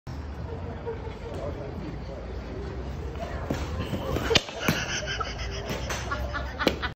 Woman golf swing fail at driving range
A woman tries to make a golf swing at a driving range tee but flips around and falls down on the ground. Took place in Deer Park, New York.